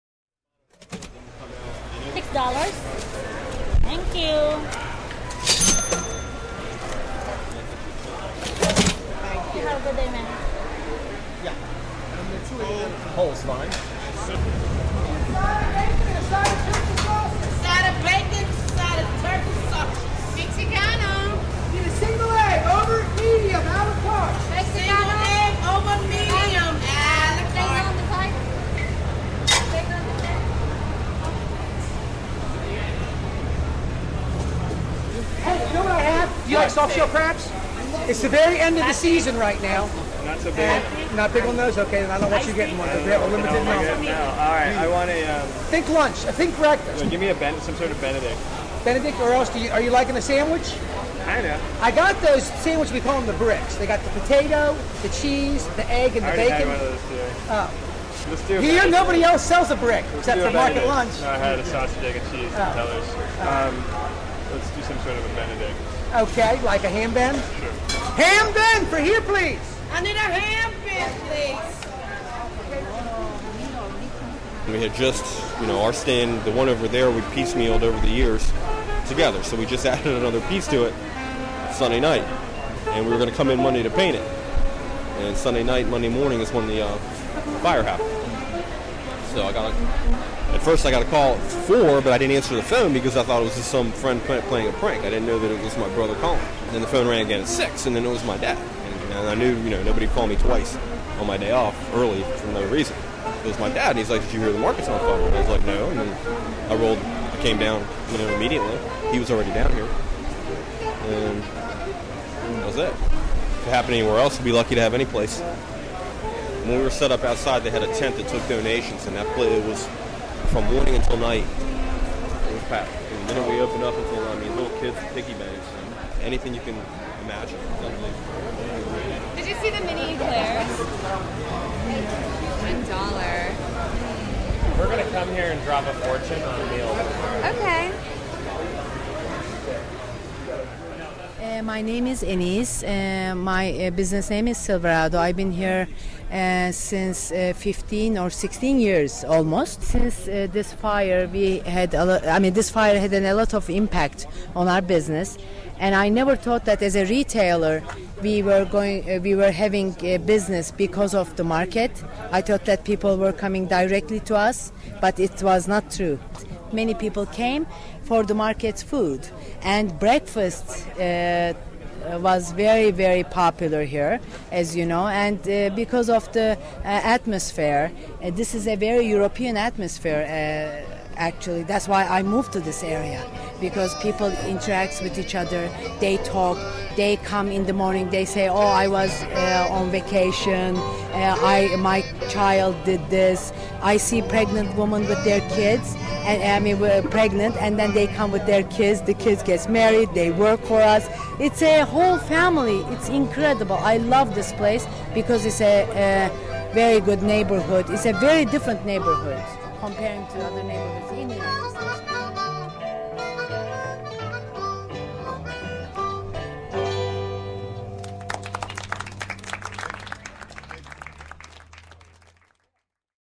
Eastern Market
Devastated by a fire earlier this year, Eastern Market on Capitol Hill has re-opened in a temporary facility, and is again attracting big crowds.